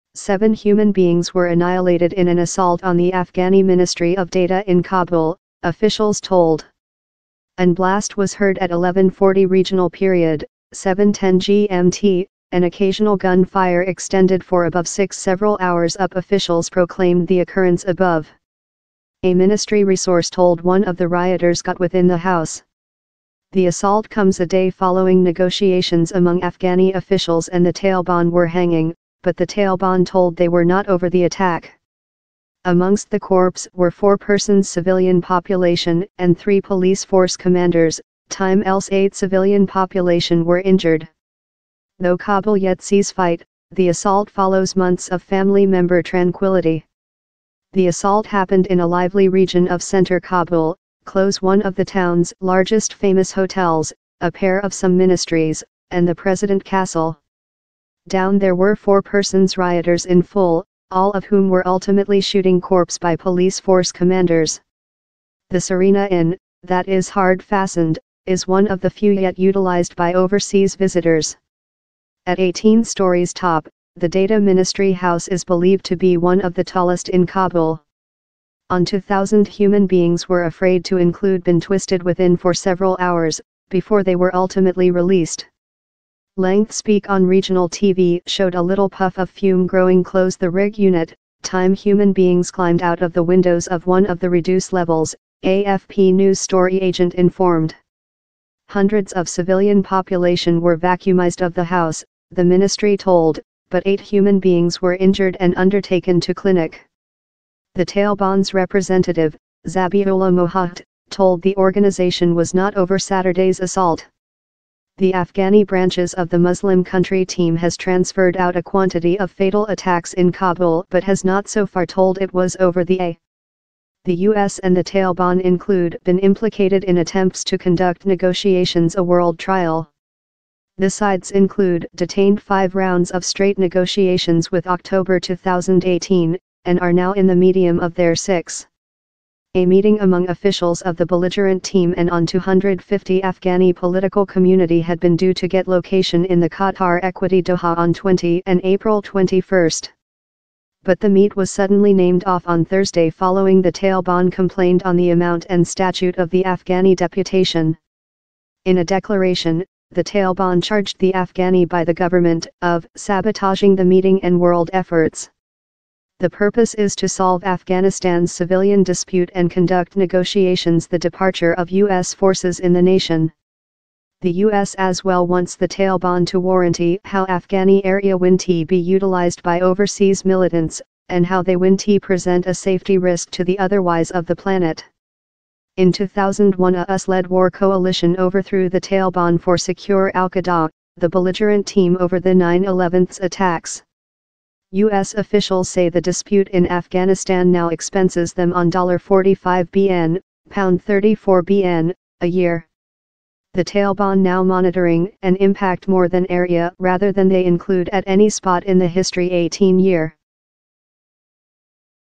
Kabul Assault: Seven Annihilated At Afghani Ministry Of Data
Four civilians and three police officers were killed in the attack, which officials say is now over.